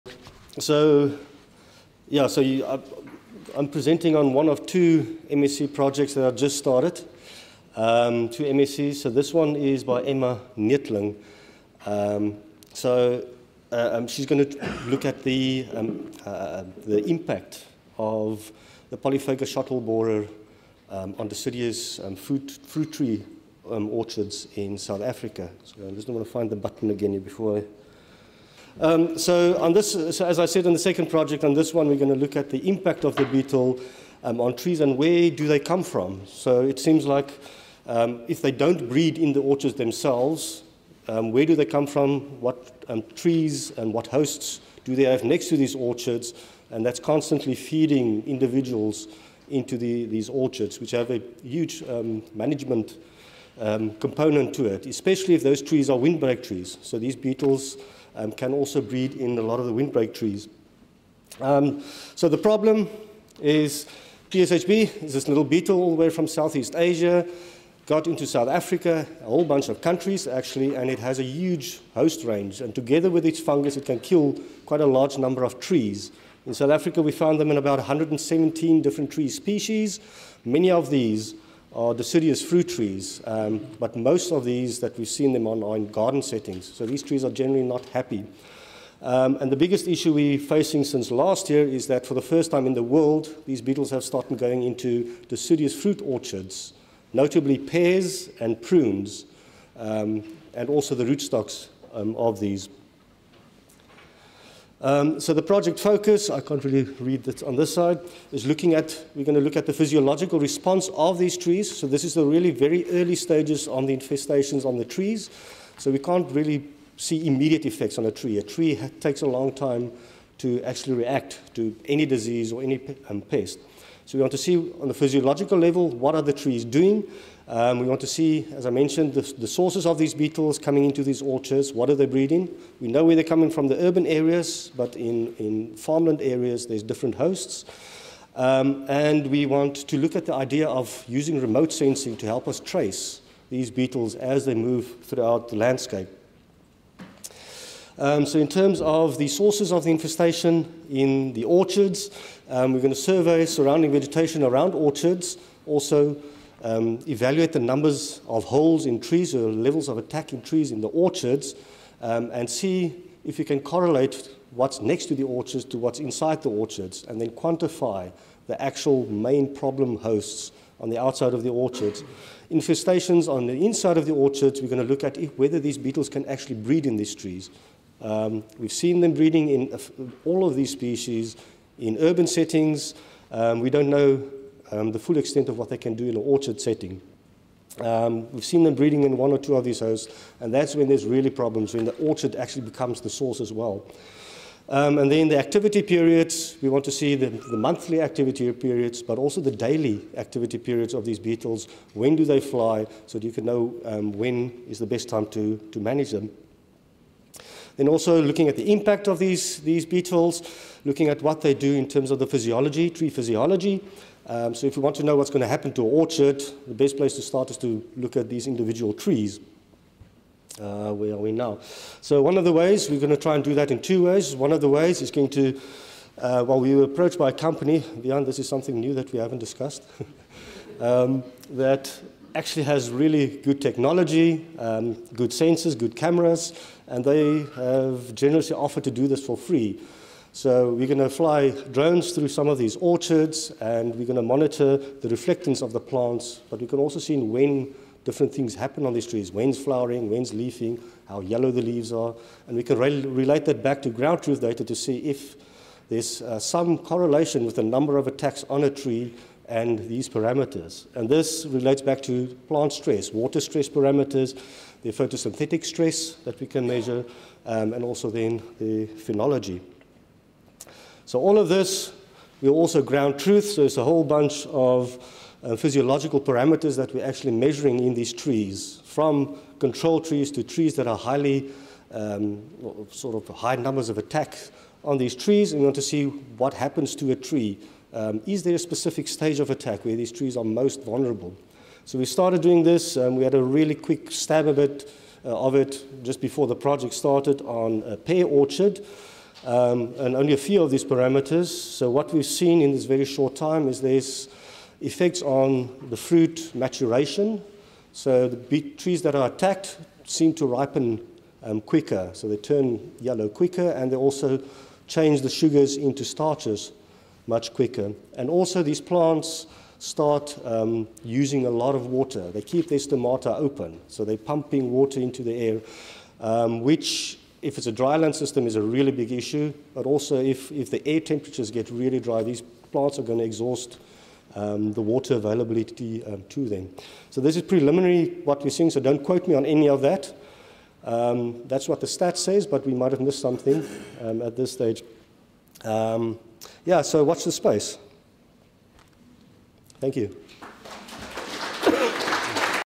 Assessing the threat of PSHB and its symbolic fungus to deciduous fruit trees
CROP PROTECTION | Integrated Pest Management | Prof Francois Roets | Assessing the threat of the polyphagous shot-hole borer beetle (PSHB; Euwallacea fornicatus) and its symbolic fungus (Fusarium euwallaceae) to deciduous fruit trees in the Western Cape, South Africa.